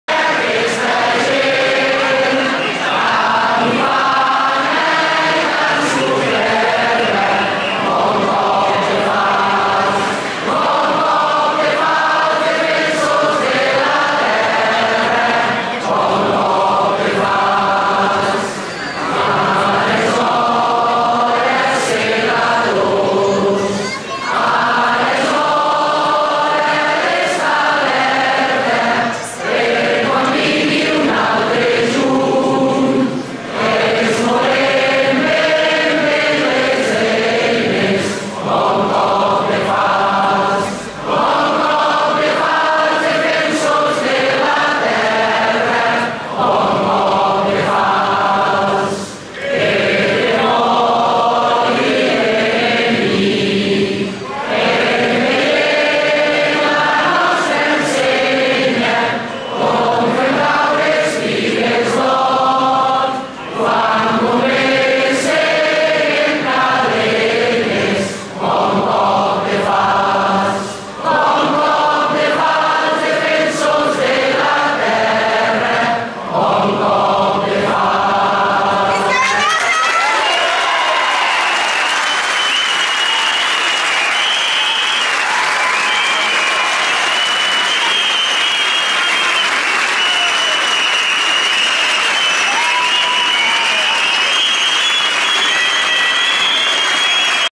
Concentració en la plaza de la Paeria a Lleida para exigir la libertad de exconsellers presos
Concentración en la plaza de la Paeria a Lleida para exigir la libertad de exconsellers presos